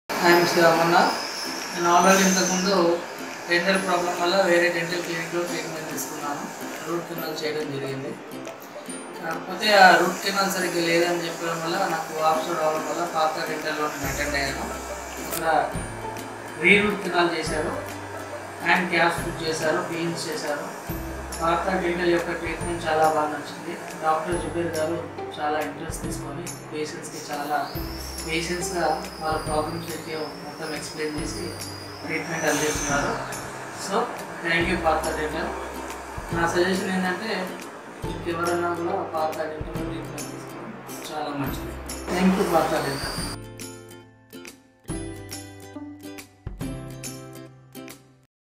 Happy Patient Testimonial After Root Canal Treatment II Partha Dental II
What occurs amid root trench treatment? ... There's no should be concerned if your dental practitioner or endodontist endorses a root trench methodology to treat a harmed or unhealthy tooth. ... Sparing the regular tooth with root trench treatment has numerous focal points: